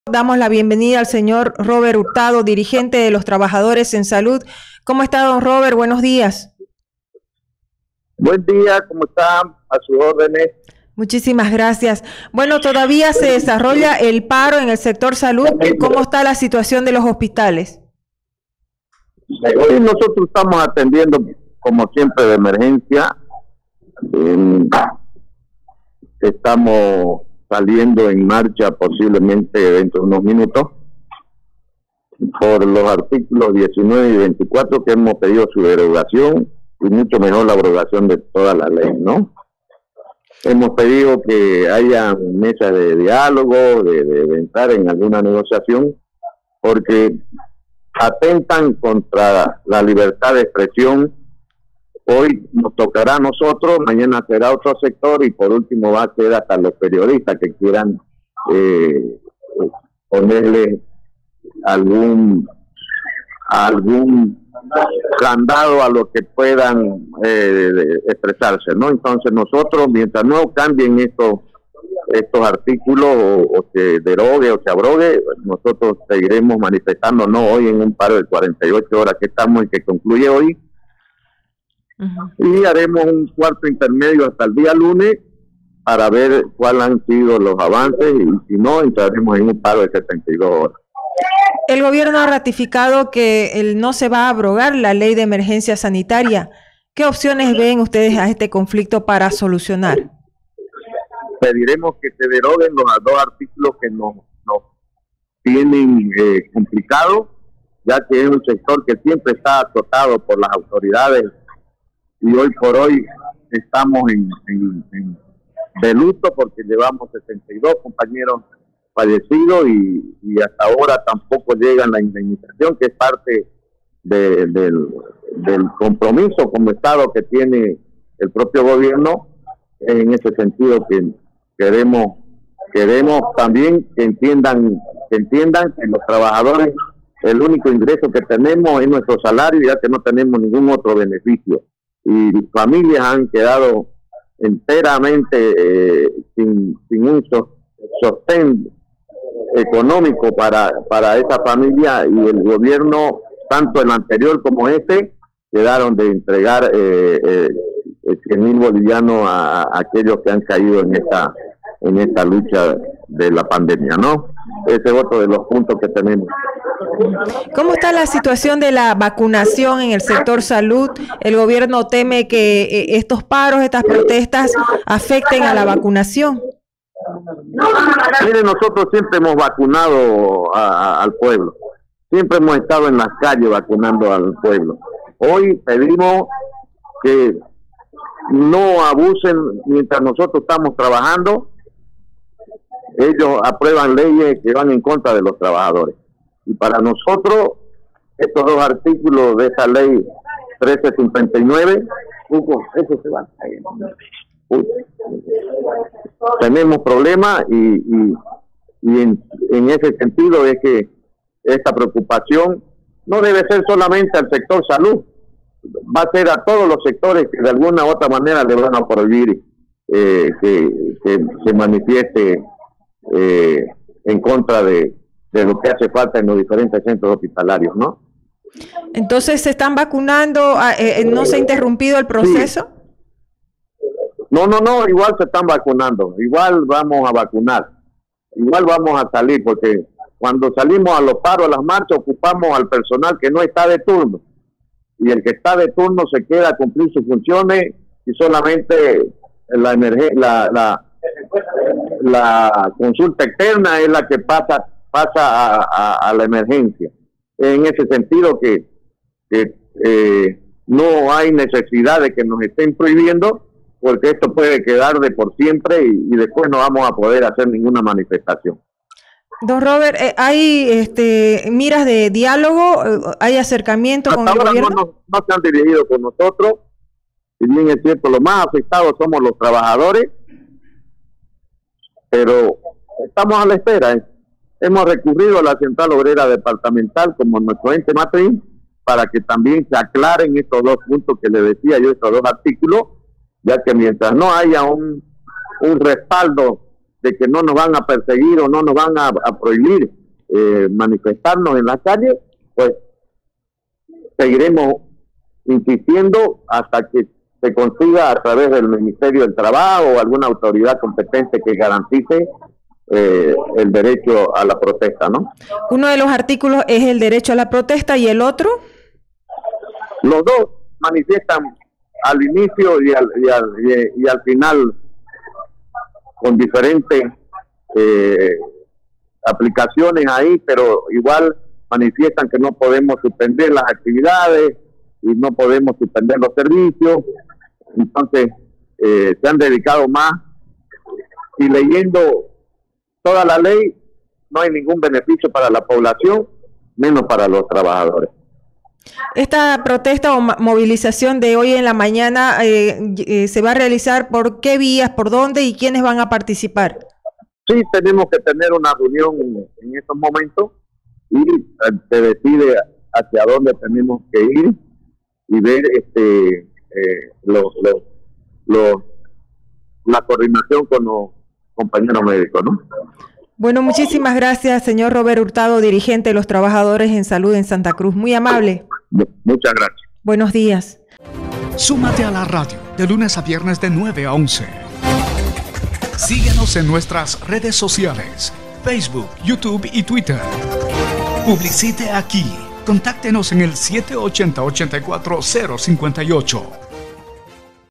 Robert Hurtado, dirigente de trabajadores en salud en Santa Cruz
TRABAJADORES EN SALUD RESPONDEN AL GOBIERNO Y GARANTIZAN SU PARTICIPACIÓN EN LA CAMPAÑA DE VACUNACIÓN CONTRA EL COVID-19, PESE AL PARO Y LAS MARCHAS EN CONTRA DE LA LEY DE EMERGENCIA SANITARIA.